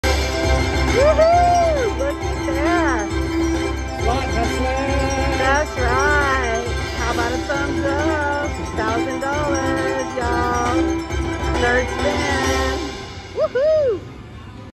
Cash Machine Won $1000 on 3rd spin- SUBSCRIBE
Thank You! Suzanne the Lady Slot Hustler